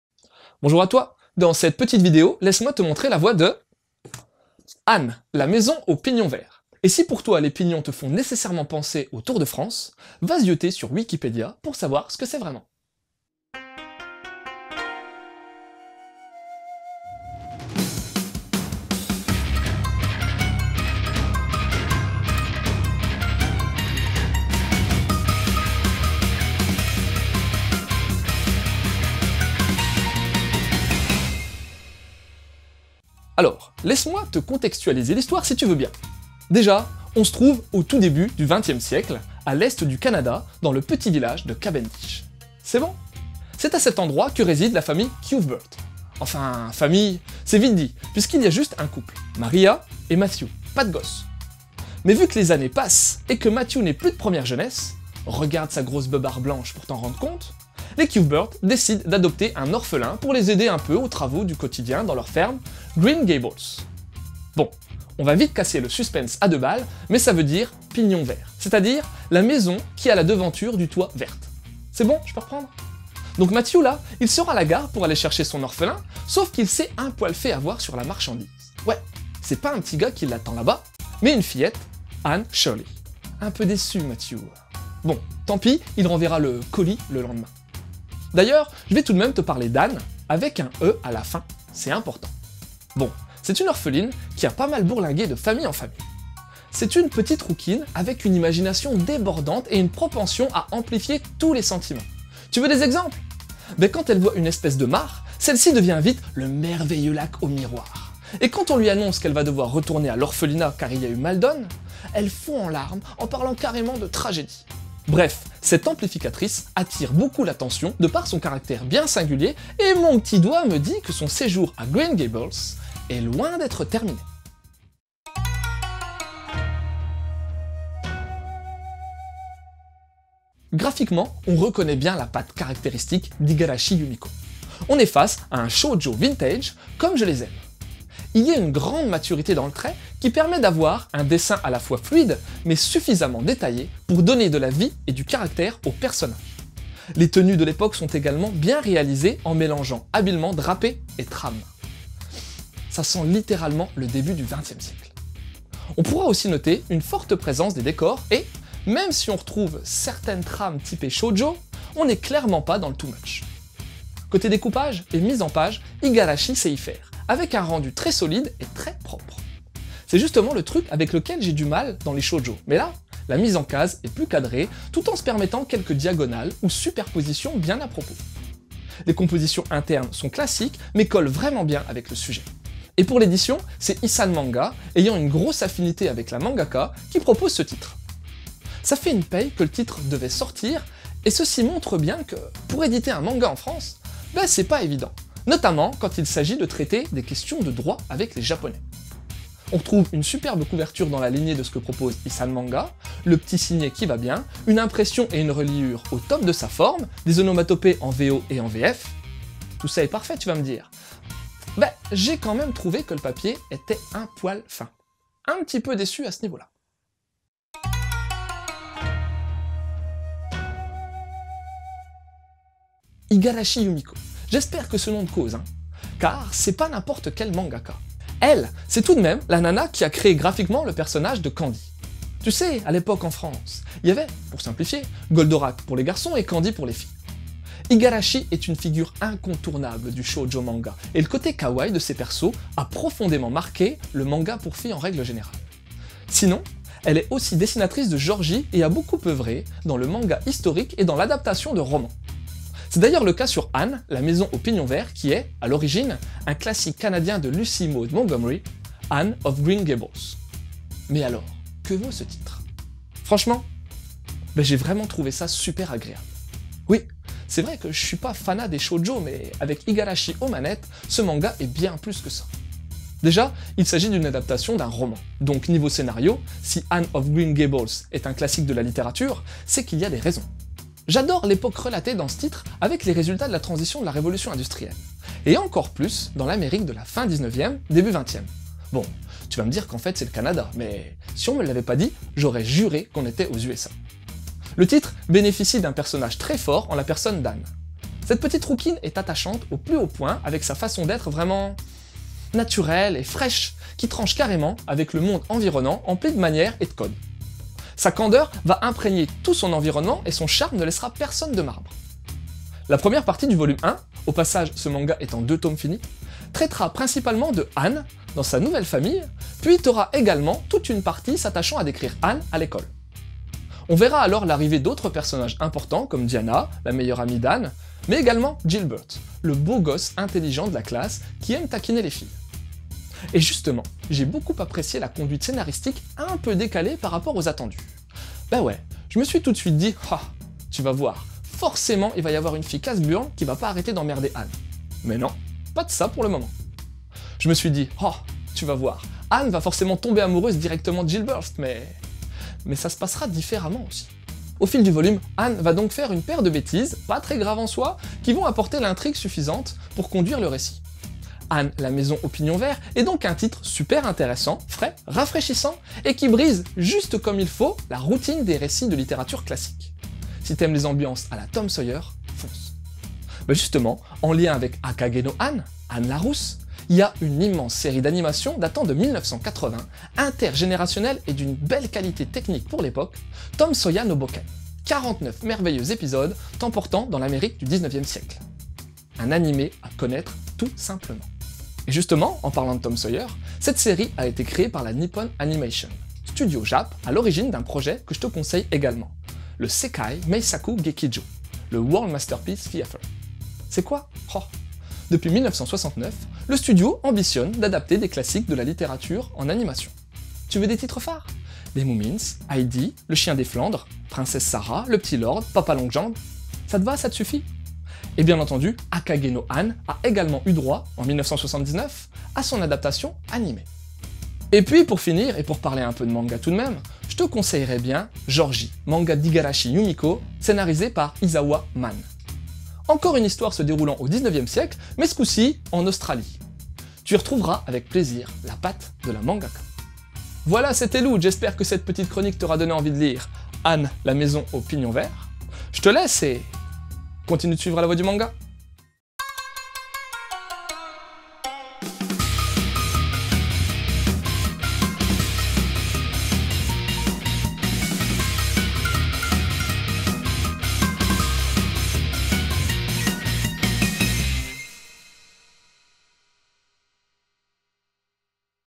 Anne et la maison aux pignons verts - Mangado La voie du manga
Voici une nouvelle fois un Mangado - La voie du manga sur Anne et la maison aux pignons verts de IGARASHI Yumiko et MONTGOMERY Lucy Maud.